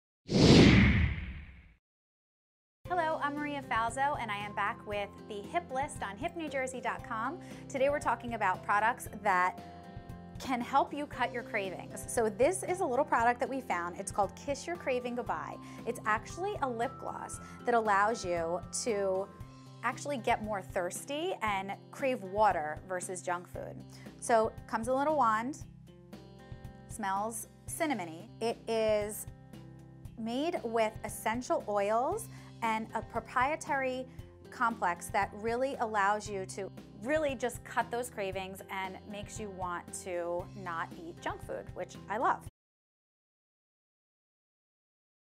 Hip Hip List - Kiss Your Cravings Goodbye
Kiss Your Cravings Goodbye is the first all-natural lip moisturizer designed to reduce cravings. Designed by the world’s top chemist, this natural treatment is based on a proprietary blend of essential oils, rare plant extracts, and hydra-infused lip conditioning agents from around the world. Within the first few applications, cravings are diminished for up to 3 hours. It will make anyone who applies it thirstier, hence making them crave water as opposed to junk food. This, along with many of Kiss Your Craving Goodbye’s products, are perfect for those willing to live a healthier lifestyle while also looking glamorous.